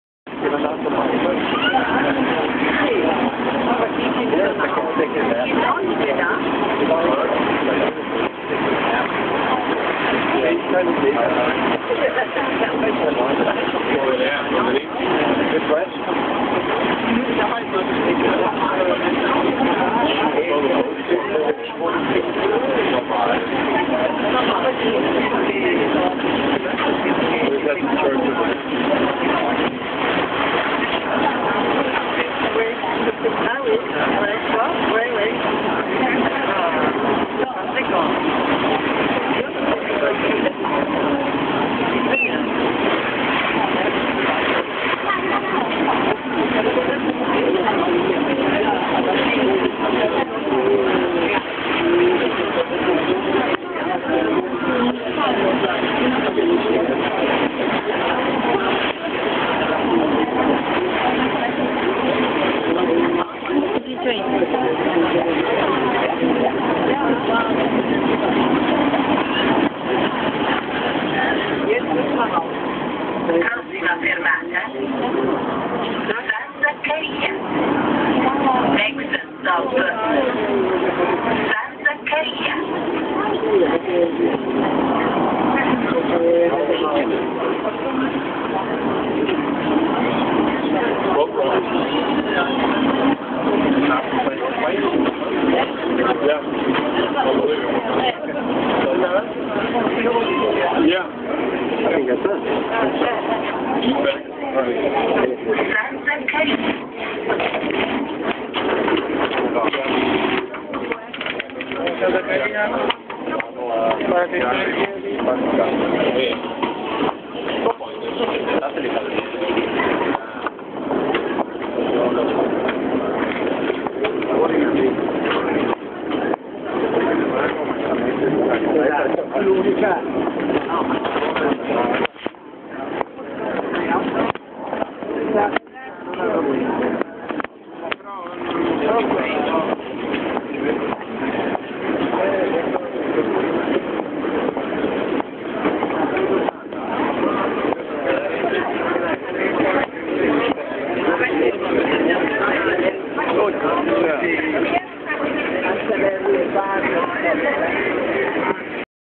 船でサンマルコへ Vaporetto to S.ZACCARIA
船でサンザッカリアへ入ると、パラッツォ・ドカーレやサンマルコ寺院が良く見えます。